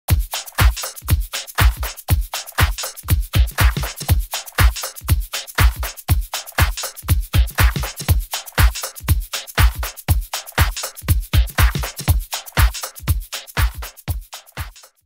Oakley Flak Jacket XLJ Sunglasses
Continue protecting your eyes in style with our Oakley Flak Jacket XLJ sunglasses! Stop in at your local Grene Vision Group for further details and Never Miss A Moment.